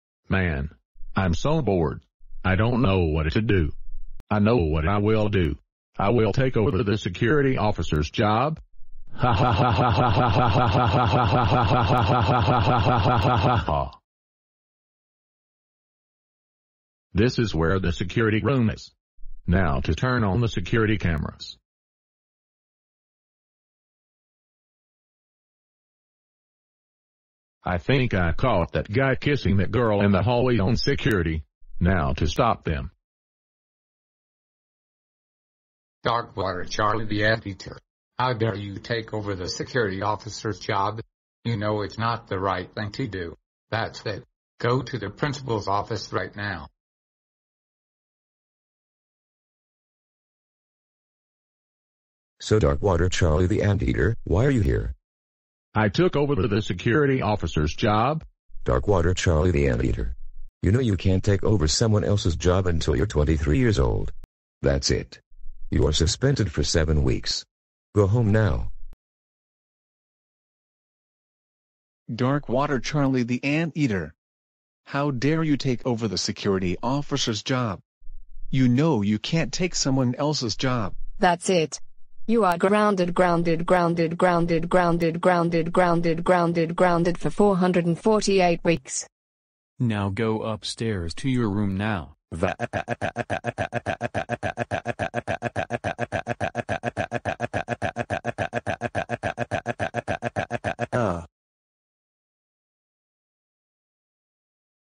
Dark Water Charlie The Anteater Takes Over The Security Officer's Job And Gets Grounded
Credit To J0N_BR0DB3CK TH3_G4M3R For The Idea